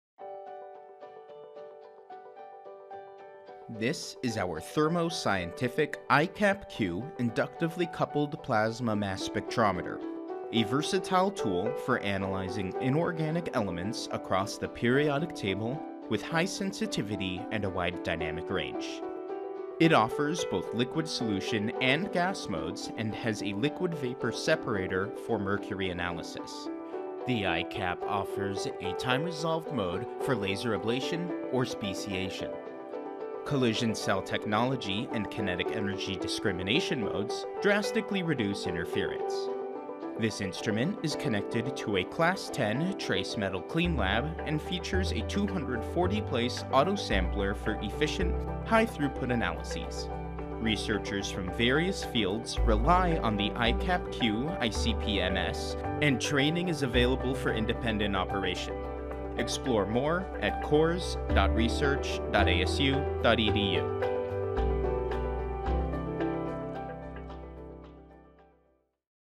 ASU Core Facilities Equipment Showcase: iCAP Q ICP-MS
The ASU Core Research Facilities house state-of-the-art equipment, including the iCAP Q ICP-MS, a tool used for analyzing elements across the periodic table.

The iCAP has Collision-Cell Technology (CCT) and Kinetic Energy Discrimination (KED) modes that reduce interference and can analyze both liquids and gasses.

As a research institution, ASU is at the forefront of innovation. Powerful equipment like the iCAP allows ASU to continue to advance research.

Learn more about the iCAP Q ICP-MS and how you could use it for your research

Information about the elements on the periodic table that the iCAP can and cannot analyze can be found

Follow the ASU Core Research Facilities on LinkedIn to keep up with our research and equipment